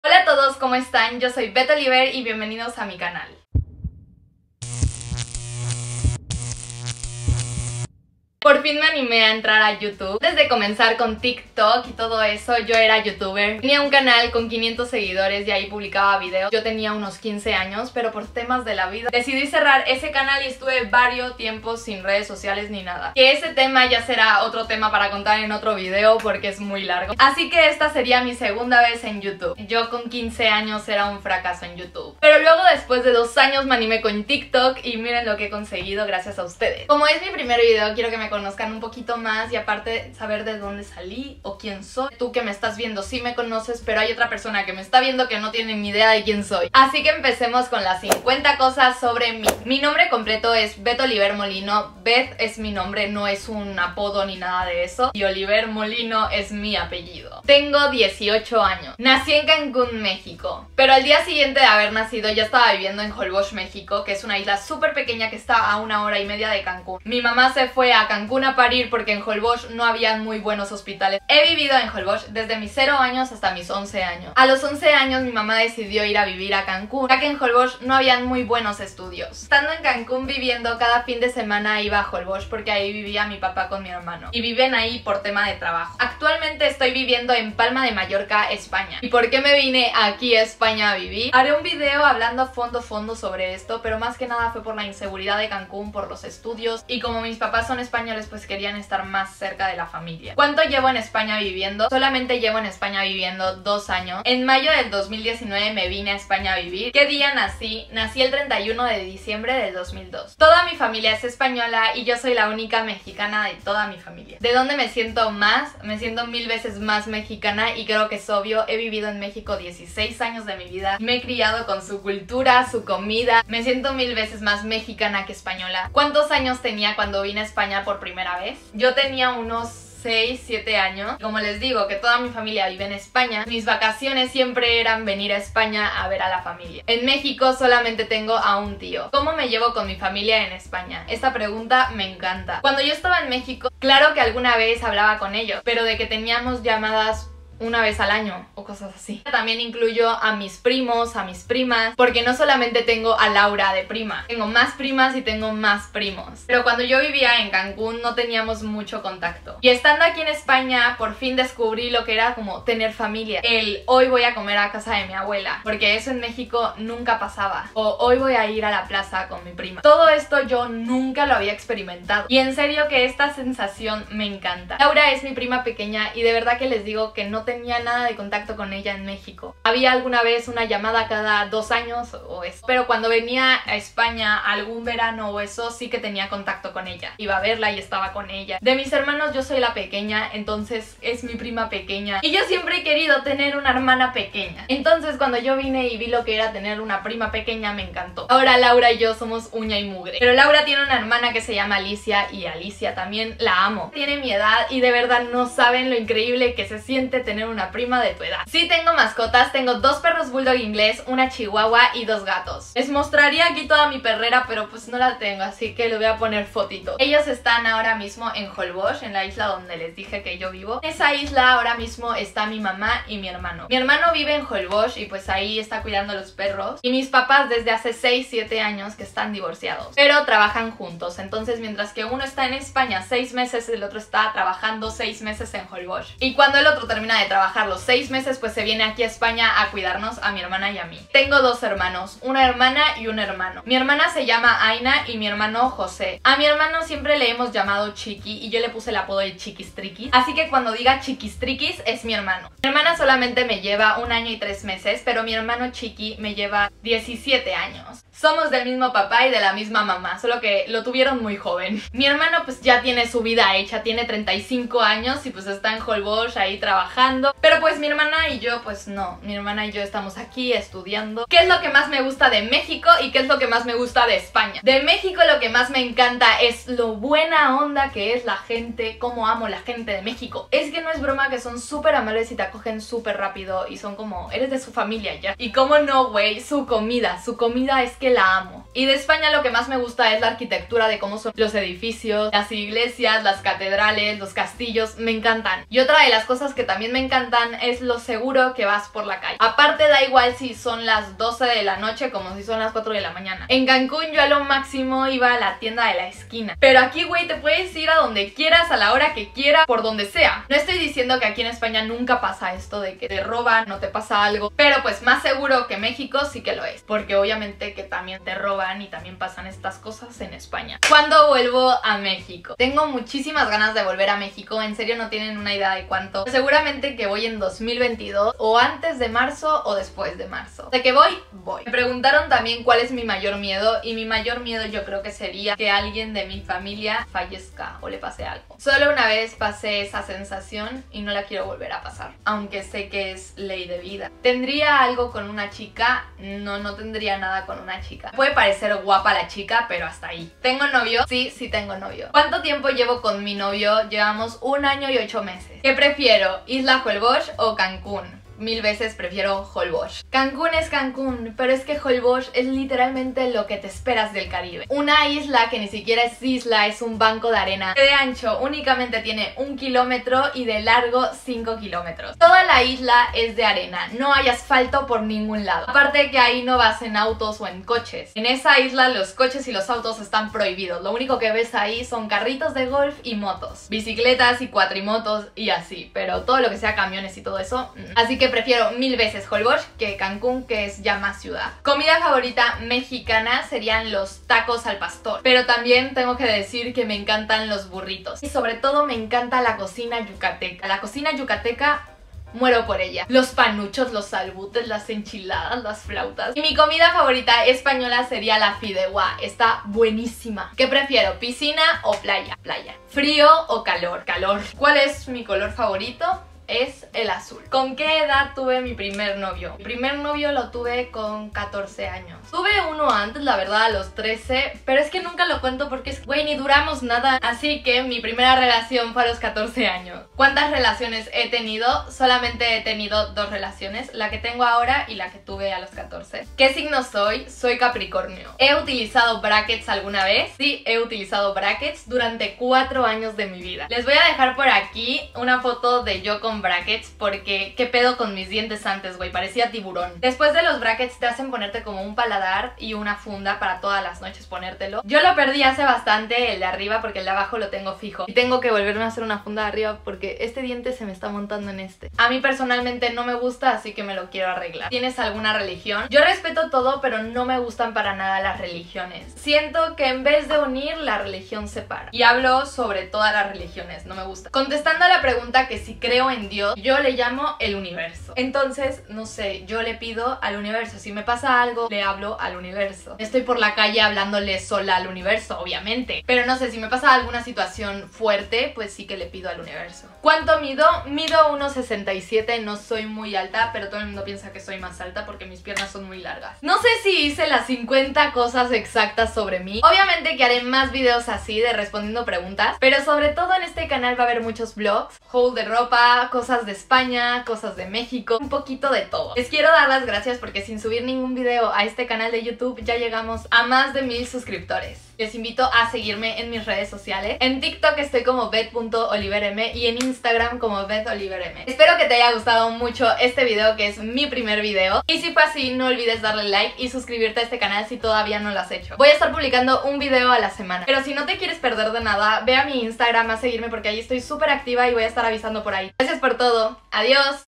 Mi primer Vídeo! ¿Porque Estoy en España? . Mexicana en España | Beth Oliver M
Porfin me anime a publicar mi primer vídeo en YouTube.
Hablaré sobre todo de España y México ya que ahora mismo estoy viviendo en España.

KWAI: Beth.oliverm